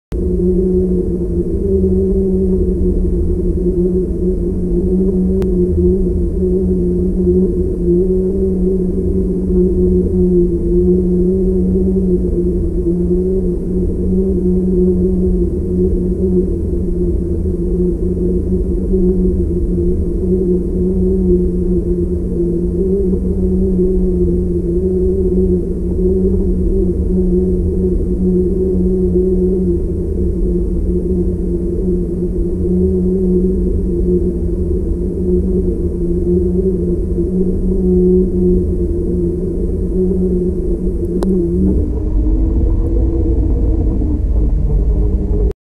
Underwater king salmon strike
Alaska King Salmon striking a hootchie